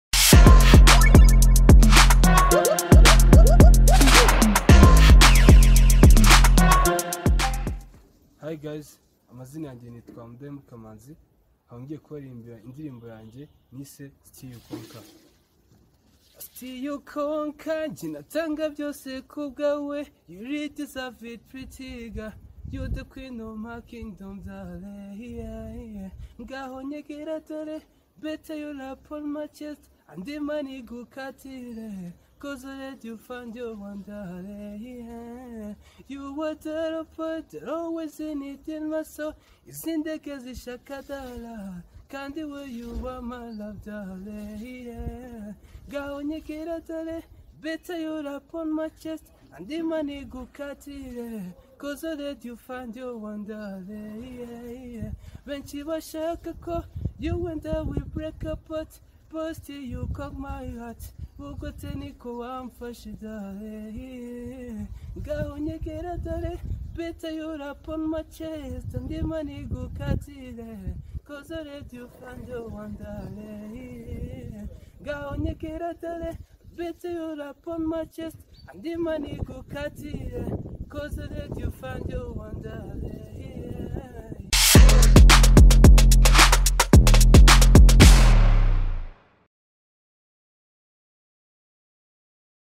MUDAHEMUKA Manzi (Musanze)
SHOW ME YOUR TALENT PART 1.

This is a monthly competition aimed at showcasing various talents from all over the country rwanda .

the competition will be held on the spot where first winners will be awarded 100,000Frw and the 2nd will be awarded 50,000frw. As part of the final 4 of them will be hosted on RTV in Versus Feed with Luckman Nzeyimana 🏆.